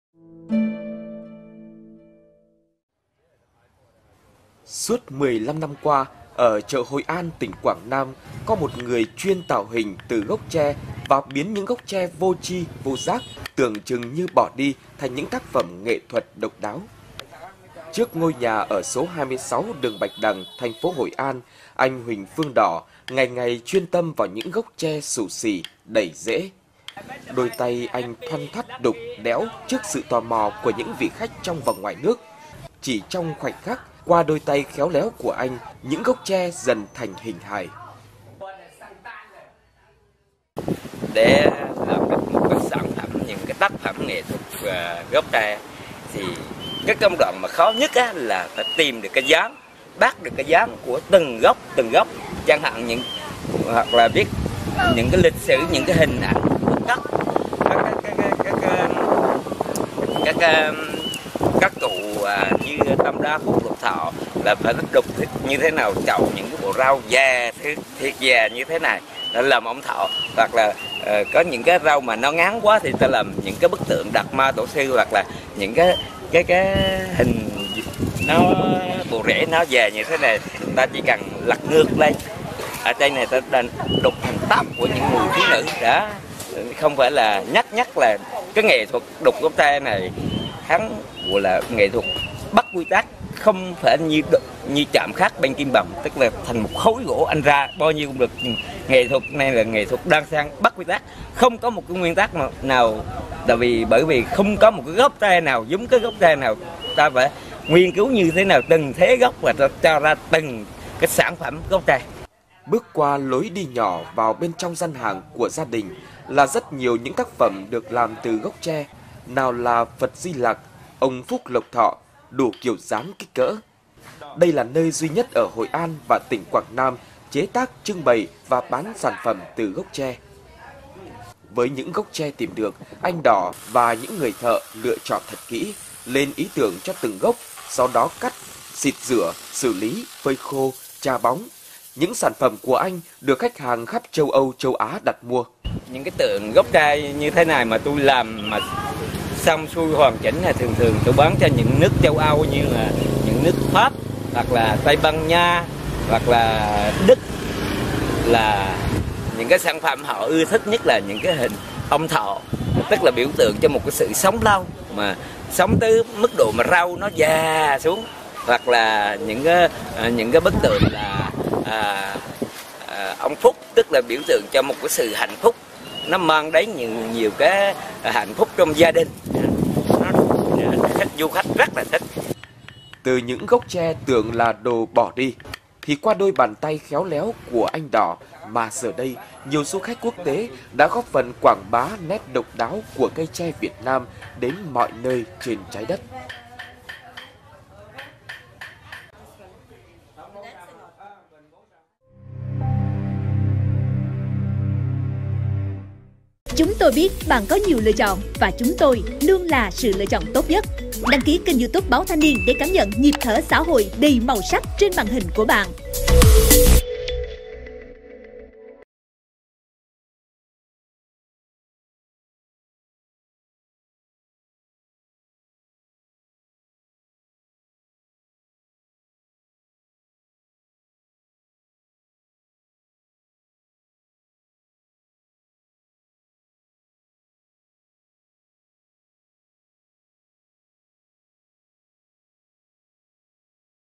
Tuyệt kỹ biến gốc tre thành tác phẩm nghệ thuật ở chợ Hội An
Suốt 15 năm qua, ở chợ Hội An, tỉnh Quảng Nam có một người chuyên tạo hình từ gốc tre và biến những gốc tre vô tri vô giác, tưởng chừng như bỏ đi thành những tác phẩm nghệ thuật độc đáo.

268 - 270 Nguyễn Đình Chiểu, Phường 6, Q.3, TP HCM